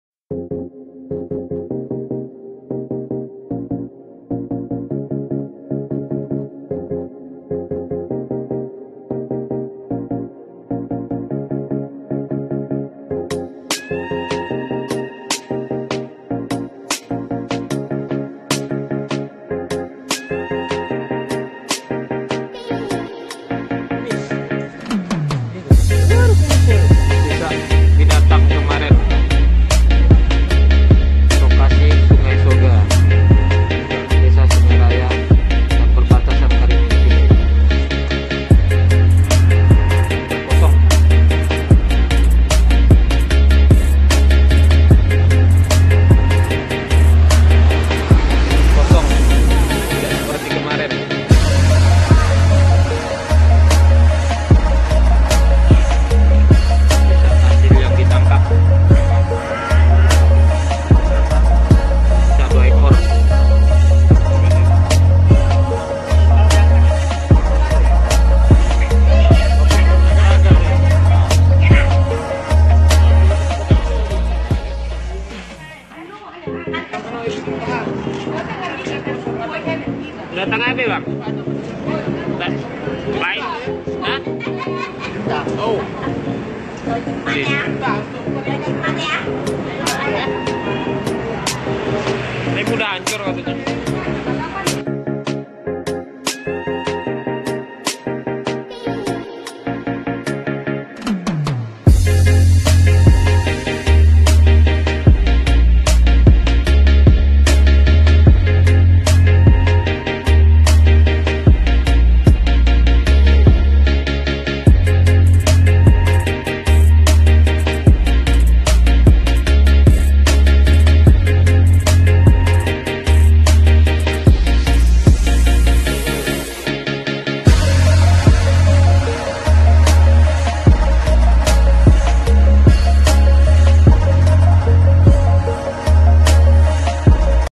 Ini Dia! Penampakan Jelas Hewan Misterius Di Sungai Raya, Bengkahyang Kalimantan Barat
Sebuah hewanmisterius tiba-tiba muncul di Desa Sungai Raya, Kabupaten Bengkayang, Kalimantan Barat.

Melansir dari Tribun Bogor, binatang itu menyerupai belut namun memiliki kaki.

Hewan itu hidup di dalam Sungai Roga dan jumlahnya sangat banyak.

Belum diketahui bagaimana kemunculan hewan misterius ini.

Yang jelas, hewan bewarna hitam itu tampak berenang memenuhi aliran Sungai Soga di desa tersebut.

Tampak di video berdurasi beberapa menit itu, warga sekitar ramai berkumpul di bantaran sungai menyaksikan kumpulan ikan itu berenang.

Menurut orang-orang sekitar, makhluk yang diduga sebagai ikan itu mengagetkan orang lagi saat diangkat dari air.

Makhluk itu tampak seperti ikan juga tampak seperti belut. Gerakannya pun tak secepat ikan dan warnanya kehitaman.

Makhluk yang namanya belum pasti itu diambil dari air.Terlihat di sana bentuknya mirip seperti kaki seribu dan juga bersisik!

Tidak seperti ikan atau belut pada umumnya, hewan itu juga tidak terlihat memiliki insang atau sisik.